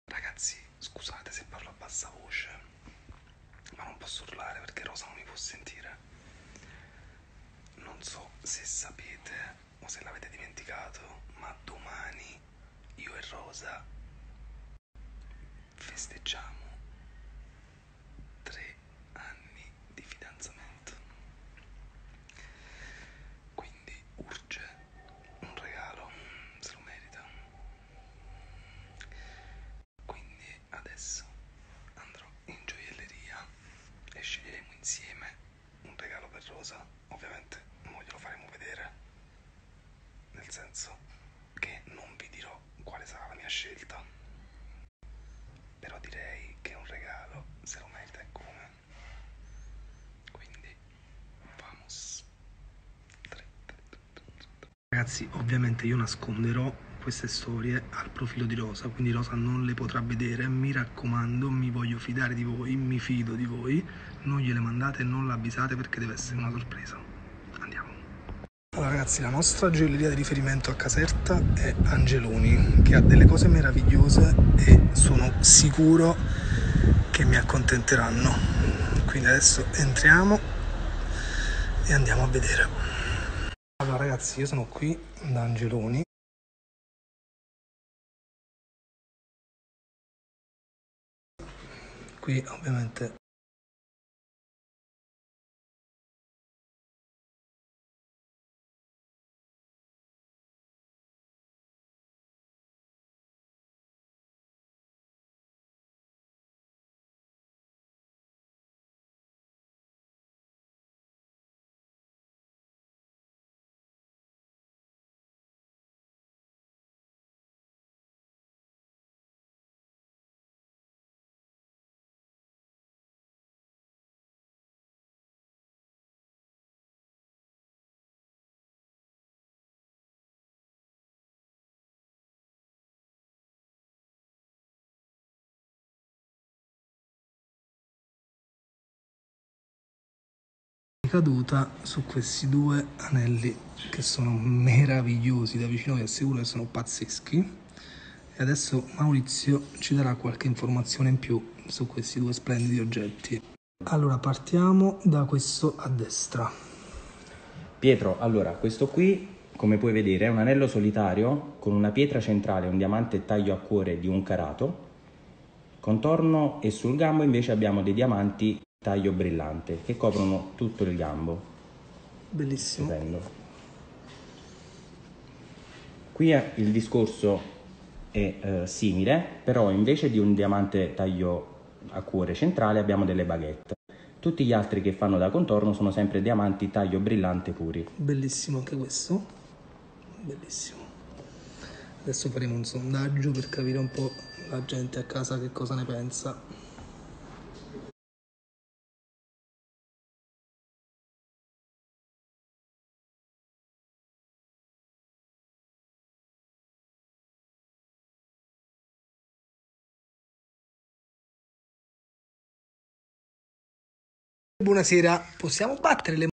PIETRO TARTAGLIONE PREPARA UNA SORPRESA A ROSA PERROTTA
Seguiteci su instagram: @gossipchannel00
Pietro Tartaglione organizza una sopresa a Rosa Perrotta per i tre anni di fidanzamento e chiede l'aiuto dei follower per scegliere un regalo alla gioielleria Angeloni.
Che romanticone.